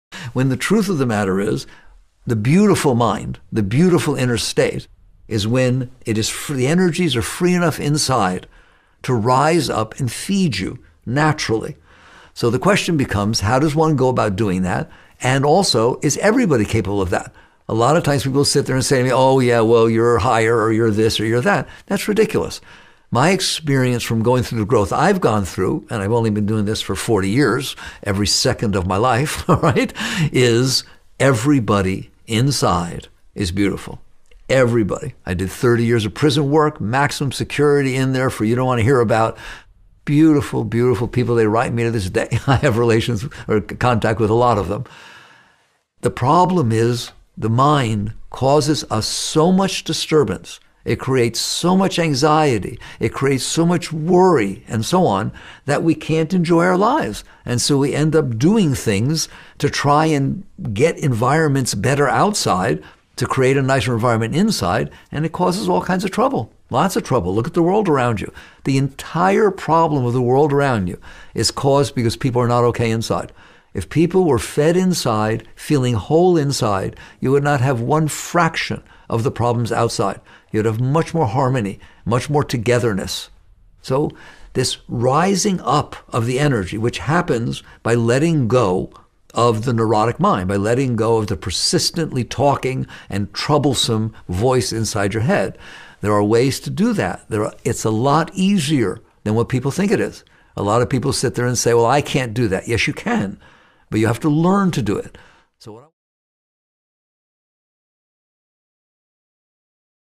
Michael Singer on freeing our mind
Get free video series "The Mind Can Be a Dangerous Place or a Great Gift" with Michael A. Singer.

Michael Singer says “In case you haven’t noticed, you have a mental dialogue going on inside your head that never stops.”

He notes this is the opening sentence of his New York Times bestselling book, The Untethered Soul.

"Since its publication in 2007, I’ve been inundated with questions about why that inner voice is always talking, why so much of what it says is negative and stressful, and most importantly, how you free yourself from it."

This video is a clip from his free Sounds True video series "The Mind Can Be a Dangerous Place or a Great Gift."